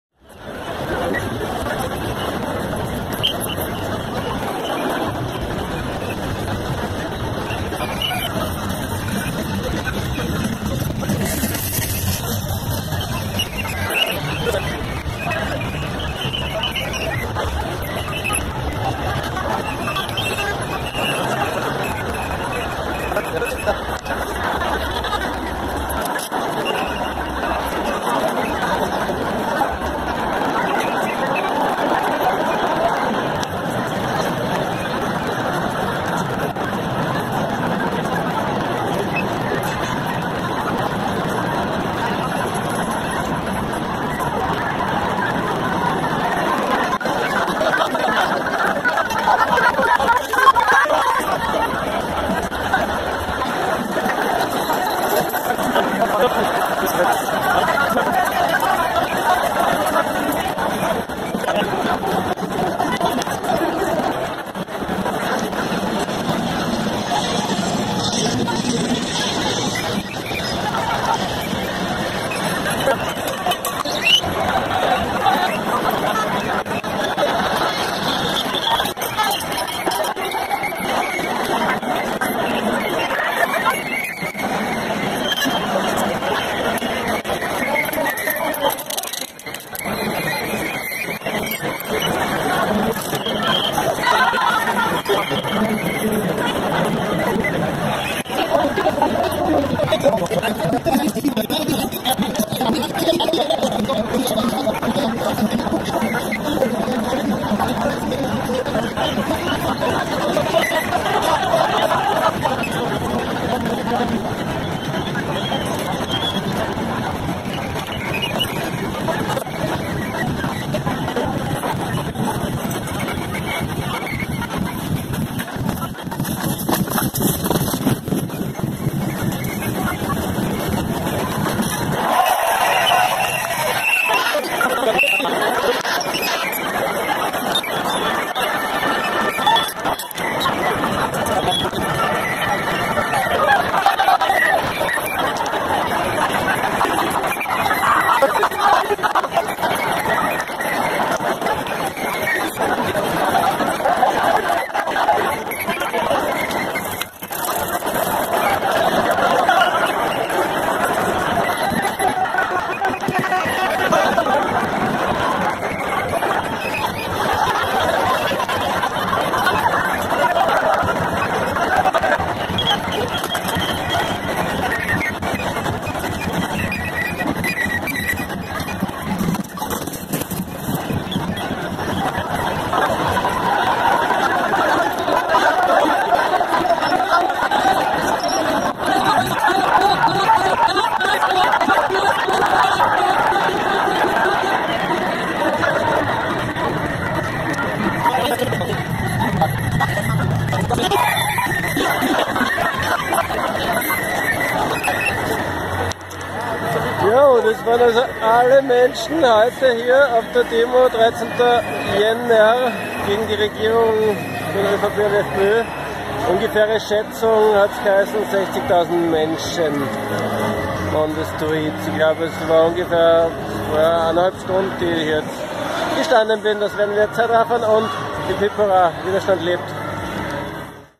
Neujahrsempfang: Großdemo gegen Schwarz-Blau! (13.1.2018)
Die ganze Demo im extremen Zeitraffer, aufgenommen Mariahilferstrasse Höhe Zieglergasse.